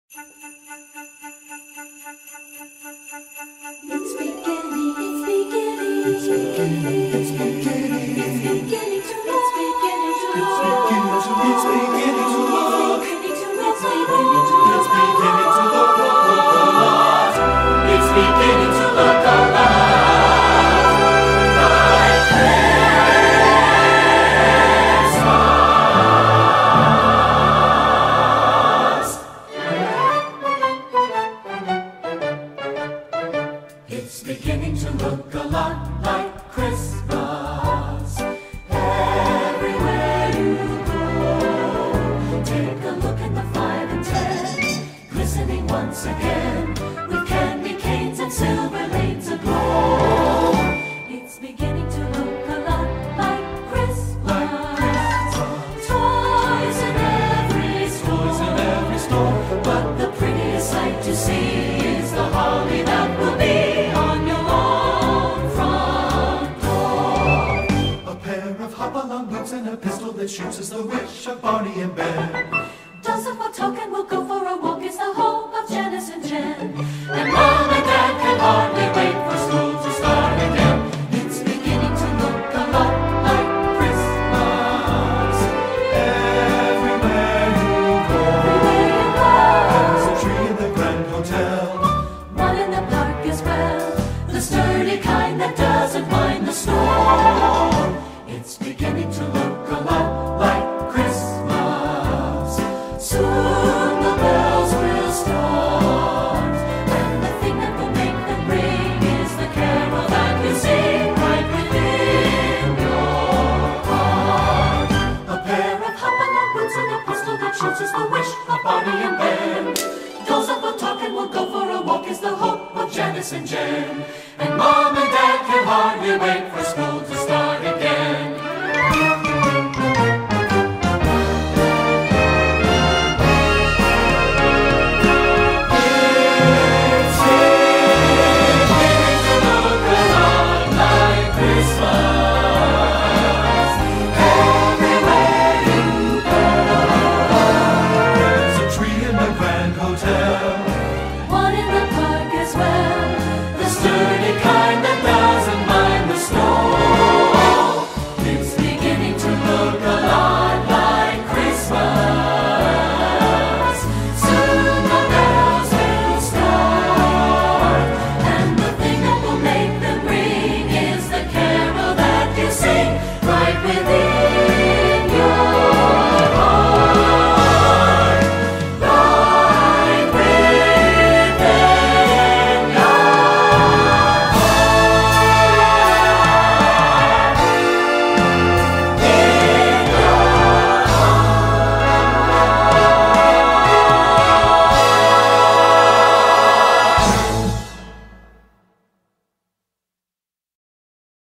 ComuSicazione Video Natale 2021
Auguri di buone feste da allievi ed insegnanti di
ComuSicazione  - Centro di Formazione Vocale e Musicale